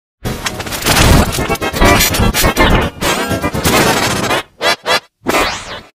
(NEW EFFECT) Klasky Csupo in G-Major 2732
How to make:

1st: Mirror - Reflect Top or (Default)

2nd: Channel Blend - RGB To BGR

3rd: HSL Adjust - Hue 0.340

4th: Color Corrector (Secondary) - Invert Color

5th: HSL Adjust - Invert Color

Pitches:
-17
-14
-12
-5
-2
+4
+7
+16
+19